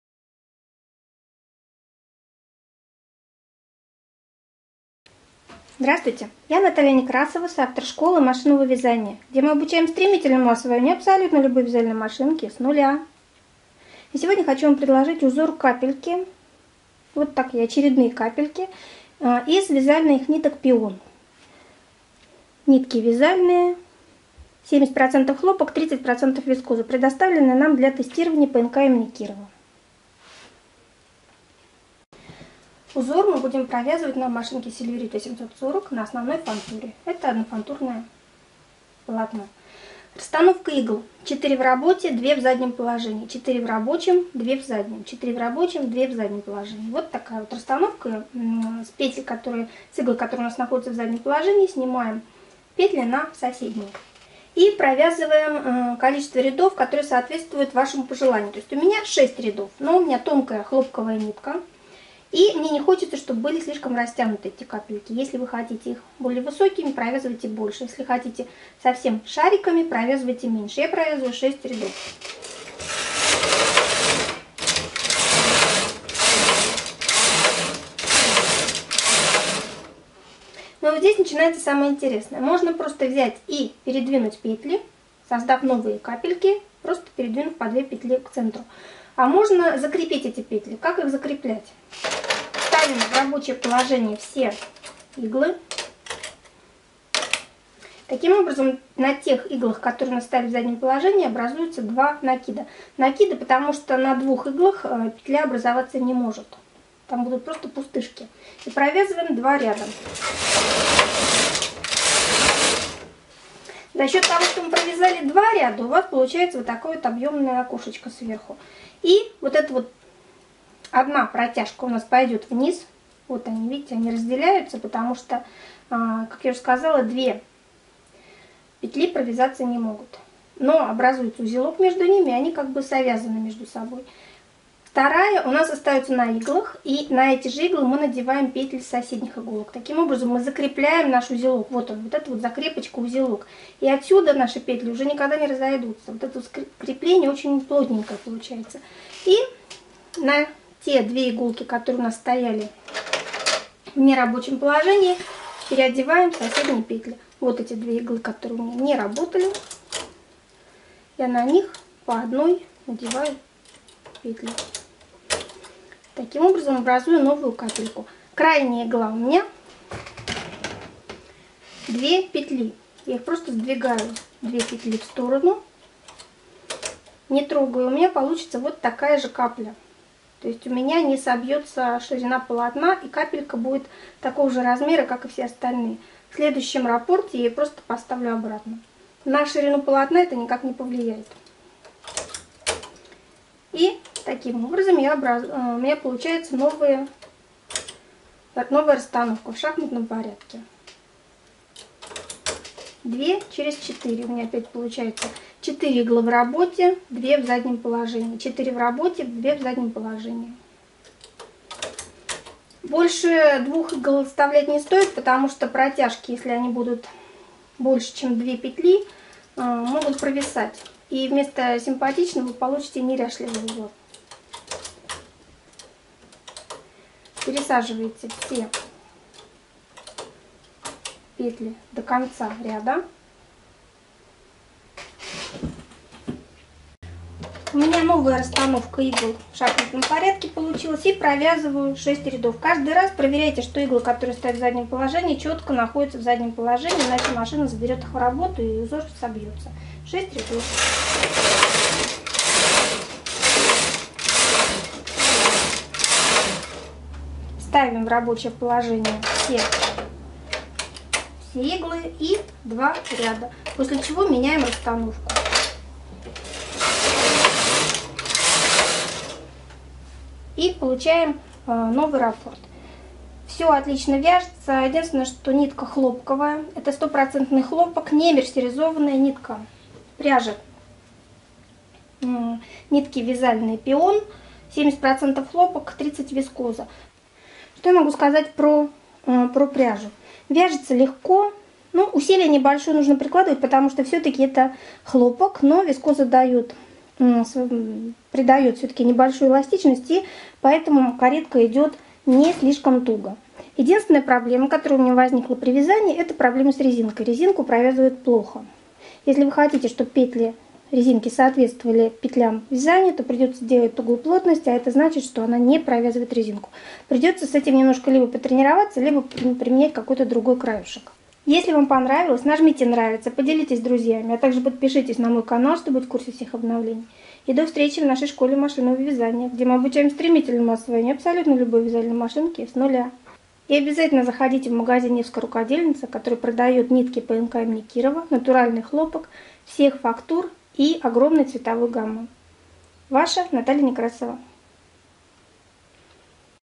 Ажурный узор машинного вязания КАПЛИ🔥Машинное вязание для начинающих🔥Узор №100
Выбирая узоры для нового летнего изделия, связаного на вязальной машине,  мы вспомнили  про ажурный узор с каплями.
Узор идеально подойдет для вязания летних изделий. Вязание на вязальной машине не вызывает трудностей. Ажурный узор очень прост в вязании и отлично подойдет и для тренировки навыков вязания  новичков машинного вязания

Мы предлагаем:
- Обучение с нуля новичков он-лайн на любых вязальных машинах: